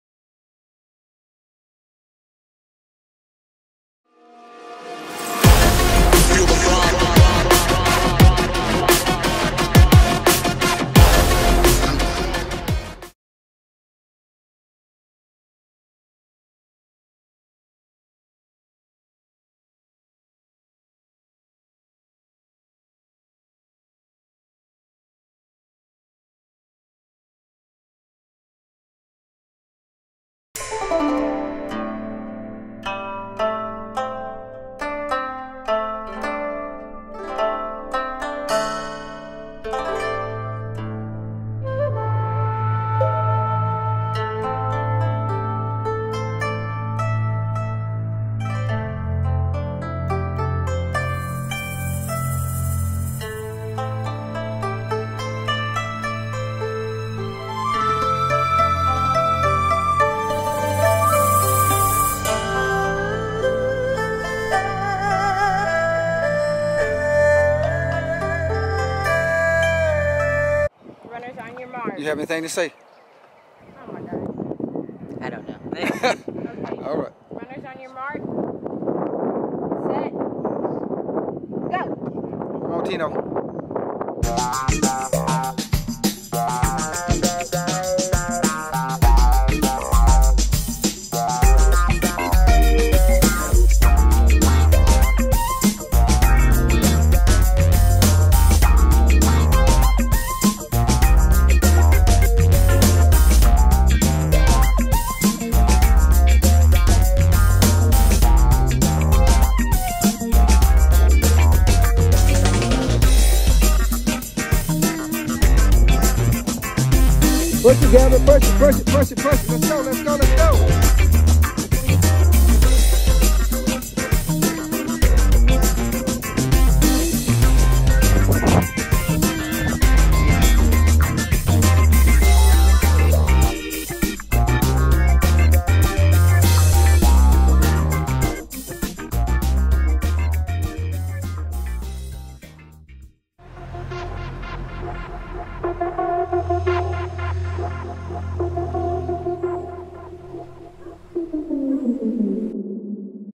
Track practice for Regionals
It's two days out from Regionals and about three week out from the TAFF track meet in Corpus, and Fantino and Gabriela is out at Fort Hood Stadium getting ready as they should, thanks to there Coach the best there is the best there was and will be thanks Coach.